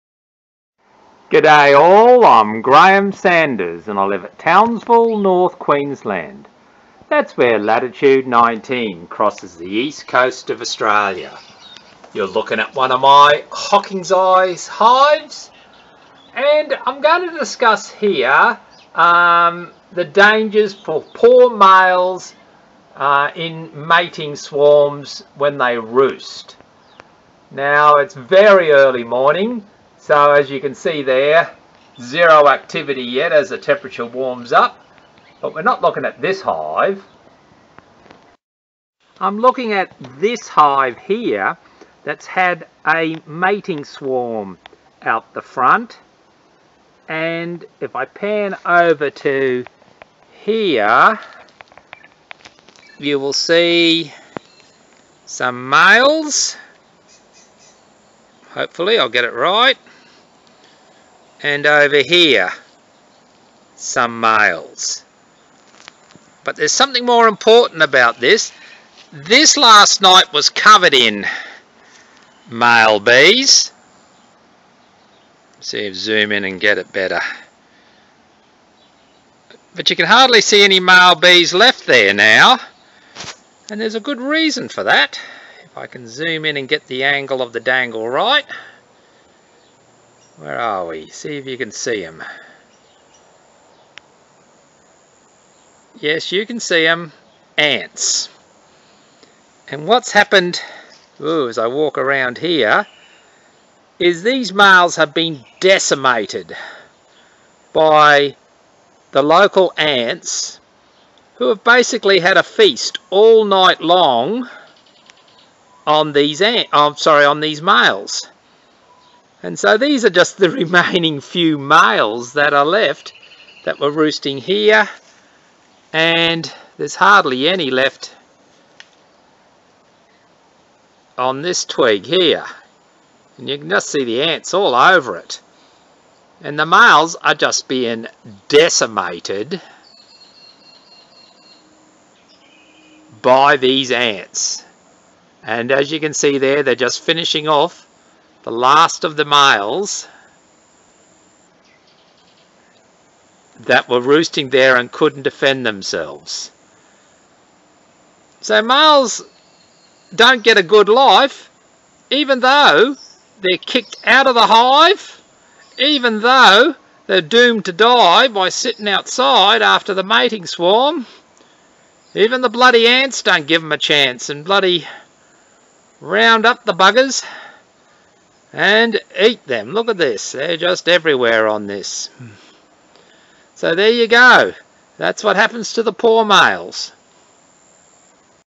North Queensland Native Stingless Bees - Disappearing Roosting Males
North Queensland Native Stingless Bees - Disappearing Roosting Males    Normally a mating swarm will last a few days, with males roosting overnight outside the hive. BUT not always. Here is an example of roosting males being attacked and killed off by ants as they roost. So no more males.